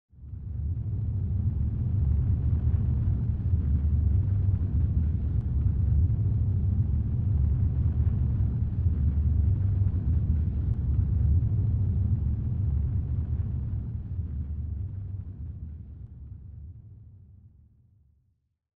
Alexandria Flyby Test 2
Test render of a starship modelled and rendered in Blender 2.5. This is an improved version with better close up detail than its predecessor.